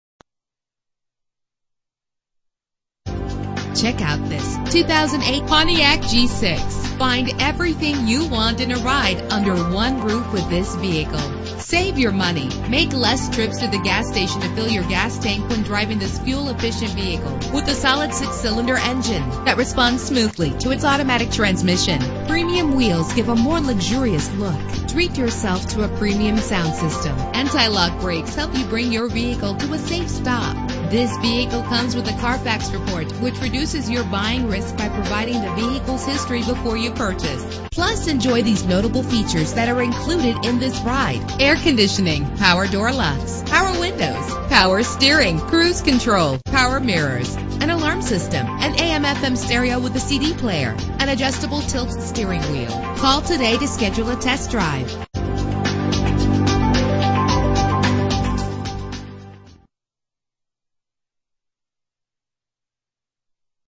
2008 Pontiac G6 - Elk Grove CA
Year: 2008
Make: Pontiac
Model: G6
Trim: GT
Engine: 3.5 liter 6 cylinder 12 valve
Transmission: 4-Speed Automatic
Color: Liquid Silver Metallic
Mileage: 45628
Address:
9776 West Stockton B